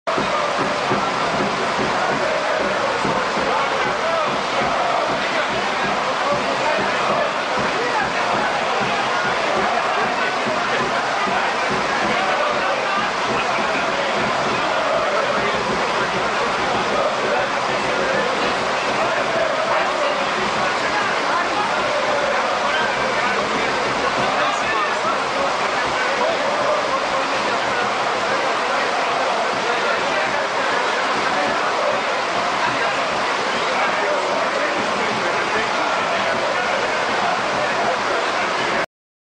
PRESSARIS: Η "κίτρινη" εξέδρα στην Ξάνθη! (2)
Η εξέδρα και τα συνθήματα των οπαδών του Άρη, στα Πηγάδια της Ξάνθης, στο εκτός έδρας παιχνίδι της ομάδας για την 20ή αγωνιστική της Σούπερ Λίγκας.